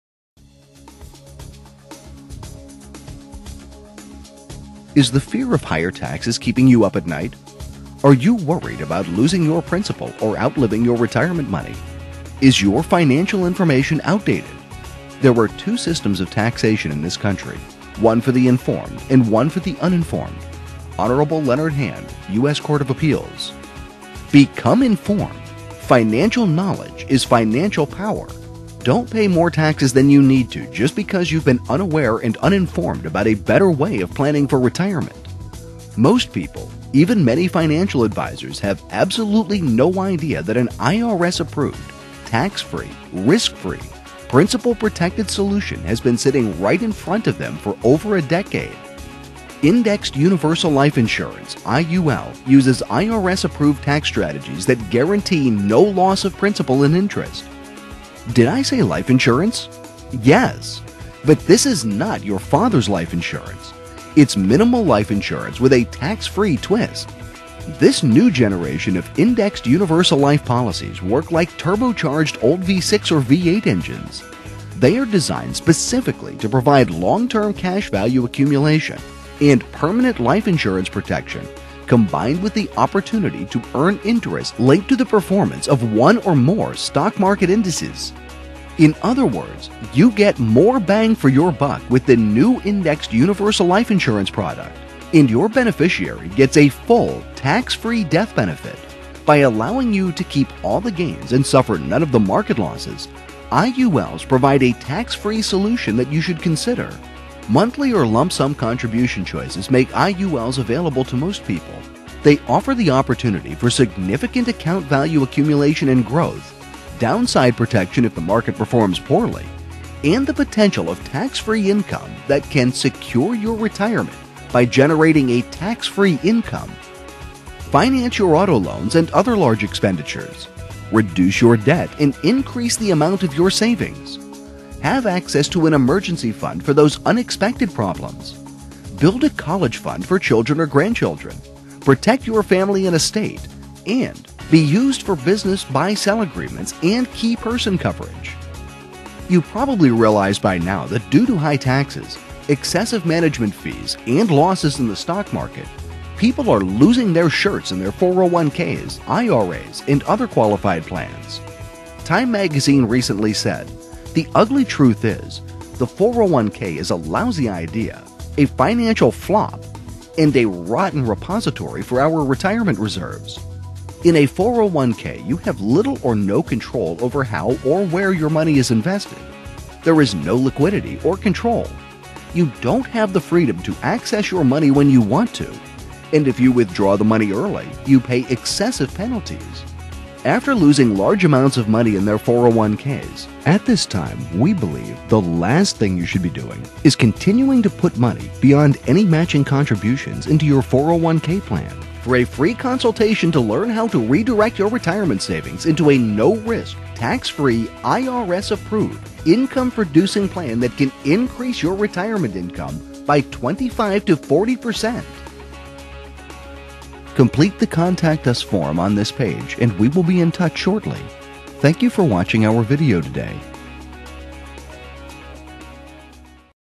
SAFE RETIREMENT thru Indexed Universal Life Insurance
There are many who WORK TOO HARD FOR THEIR money only to see it DWINDLE over the years through TRADITIONAL PLANS (401k etc.) Learn a WAY TO GUARANTEE your money AGAINST LOSSES and RETIRE TAX-FREE !!